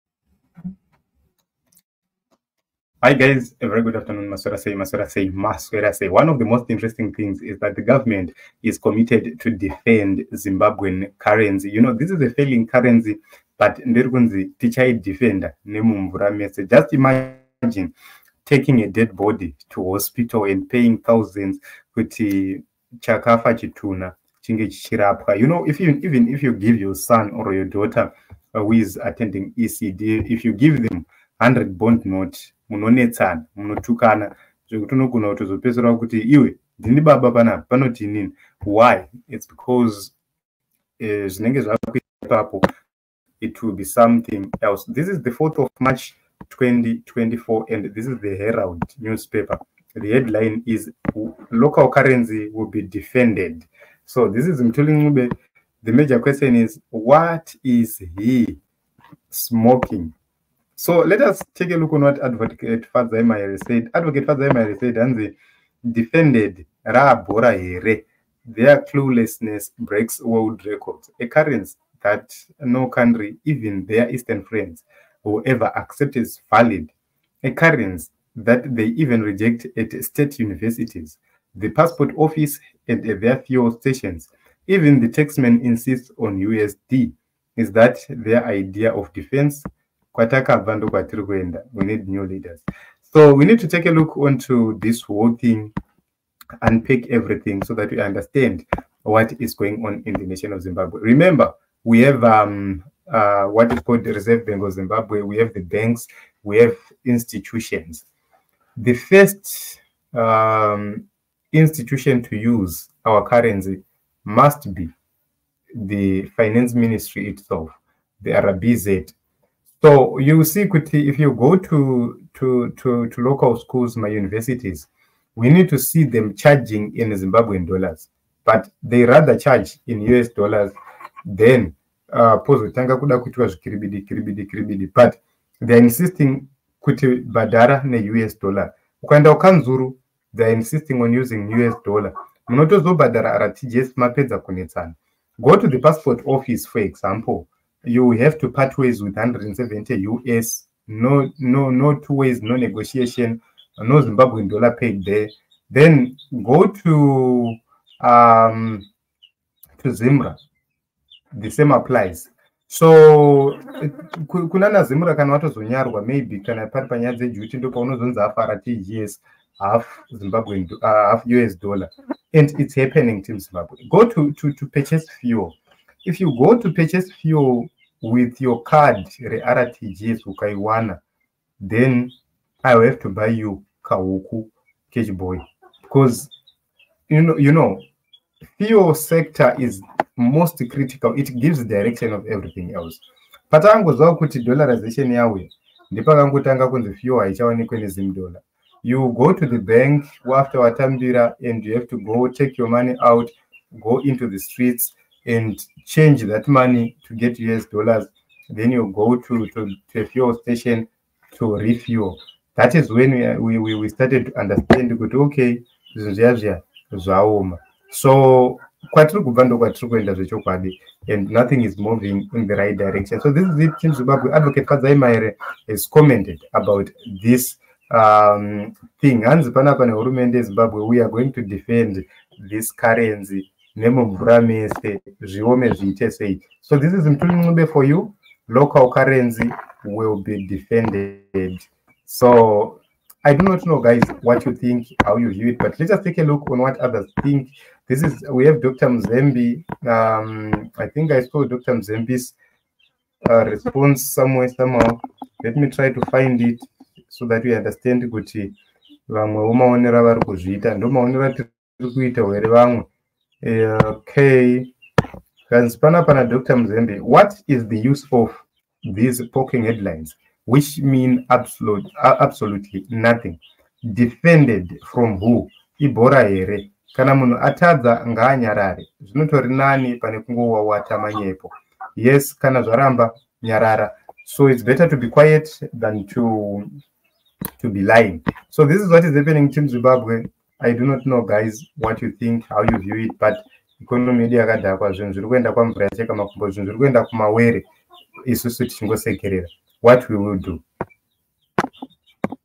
Mahere Destroys Mthuli Ncube over currency lies
Mthuli Ncube exposed on Currency lies